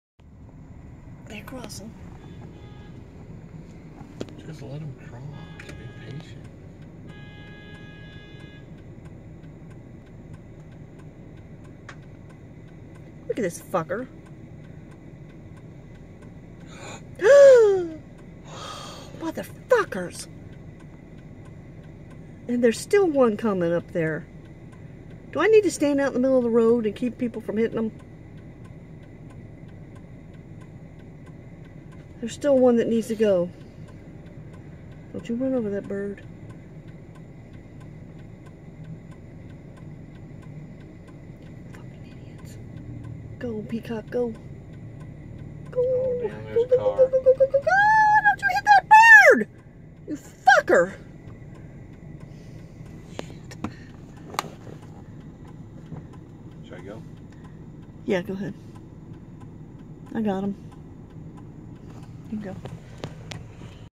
Peacock and Peahen crossing the road
This is a known wild Peacock and Peahen crossing area in Cocoa Beach Florida, and yes there are signs posted.  We are at a 4 way stop and these drivers are being asses toward these birds.  Luckily no birds were hurt when we were there.